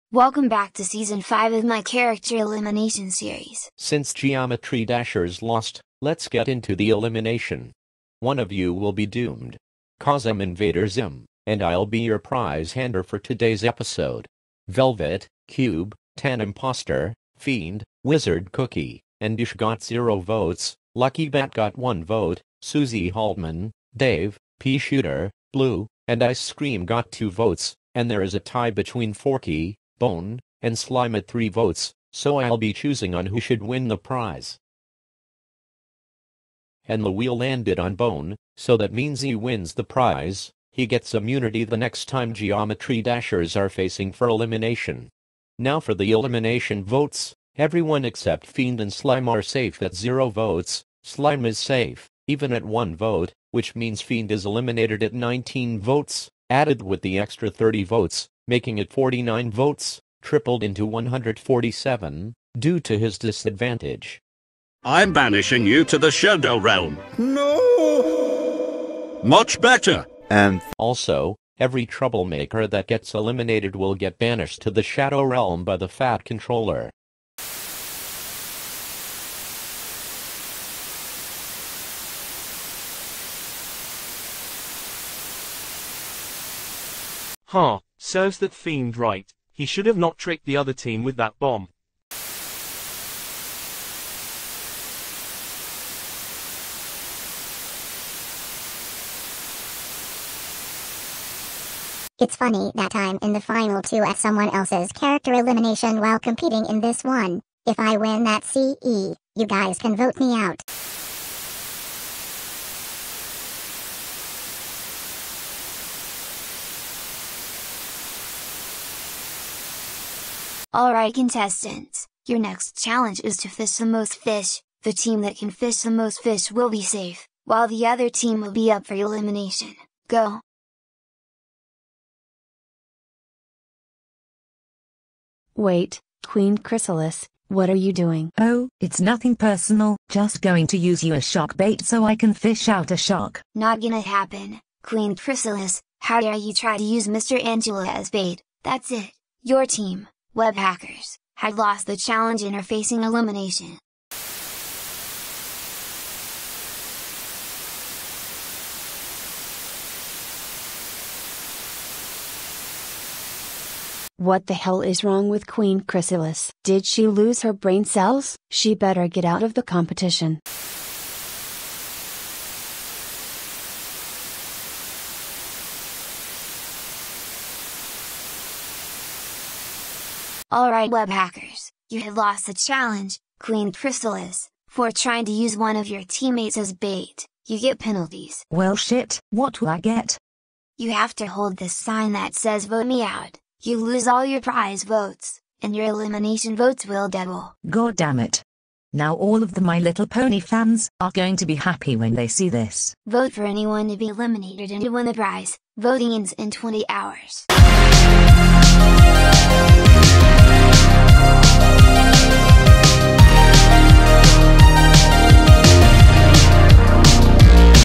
Character Elimination Season 5 Episode 2: "Fish n Chips" (Voting Closed)
The contestants must fish